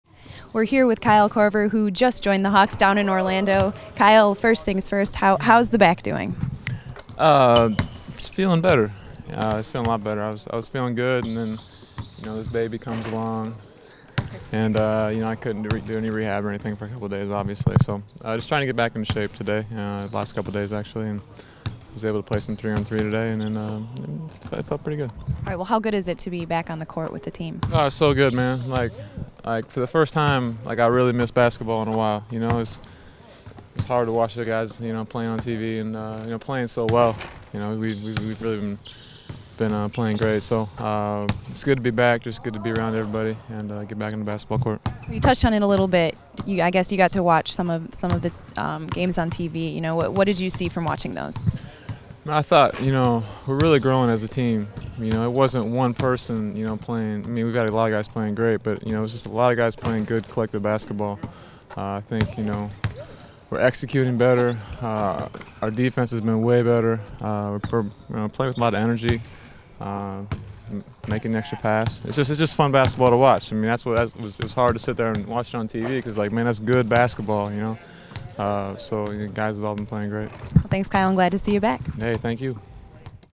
Kyle Korver returns
Kyle Korver returns to Atlanta Hawks practice after missing time with back spasms and the birth of his daughter.